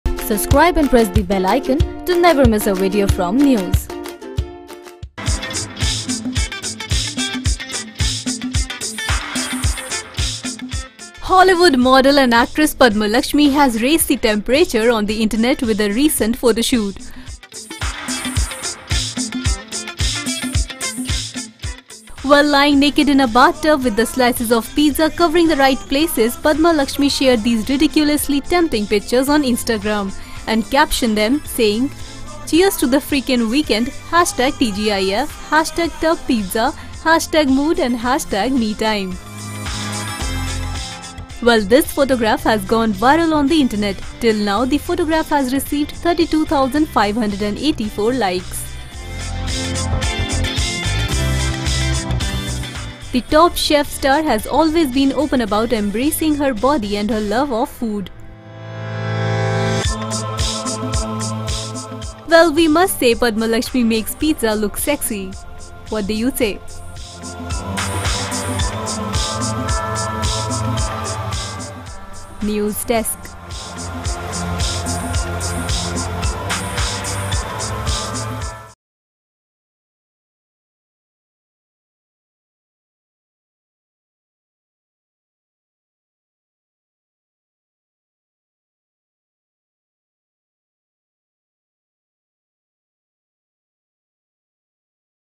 VIRAL PIC: Padma Laxmi in bathtub goes naked with pizza
Hollywood model and actress Padma Laxmi has raised the temperature on the internet with her recent photoshoot.
Well, lying naked in a bathtub with the slices of pizza covering the right places, Padma Lakshmi shared these ridiculously tempting pictures on Instagram and captioned them saying, “Cheers to the freakin’ weekend TGIF tubpizza mood metime.”

This photograph has gone viral on the internet. Till now, the photograph has received 32,584 likes.
The ‘Top Chef’ star has always been open about embracing her body and her love of food.
Well, we must say Padma Lakshmi makes Pizza look sexy. What do you say?